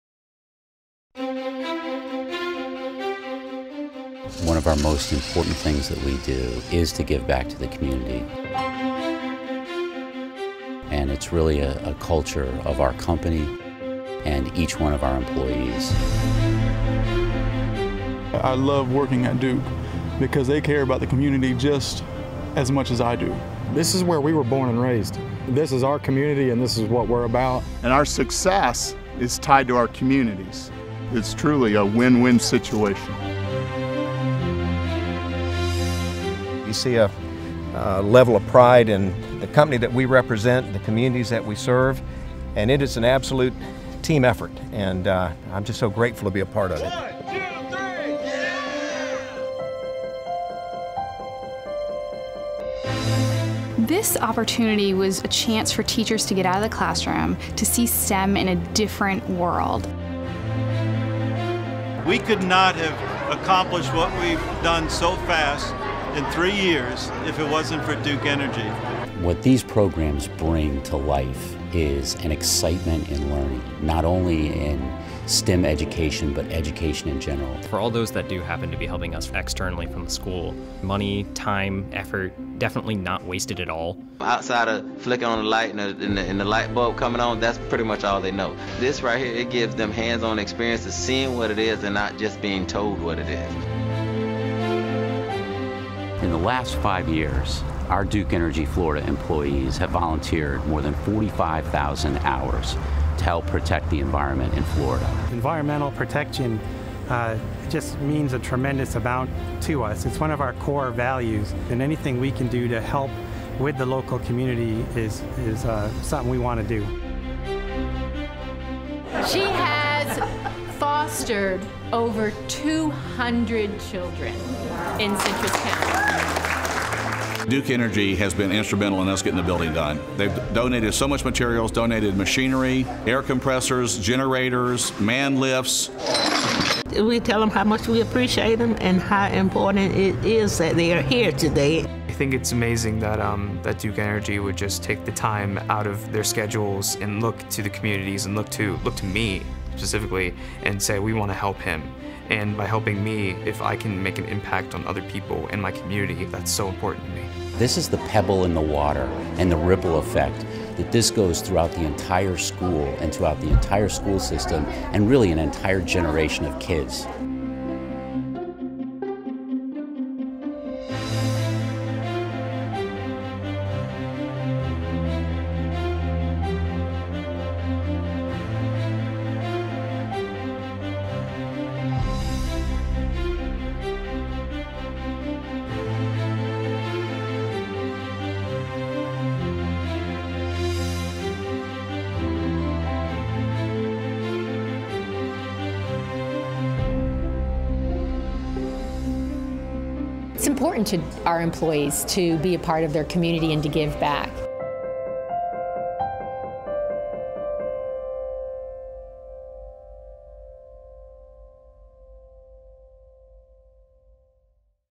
Duke Energy's Commitment to the Community
Duke Energy Florida aims to make a difference in the communities we serve. This video highlights 2016's employee volunteerism and other accomplishments in Florida, which were possible with the support of our community partners.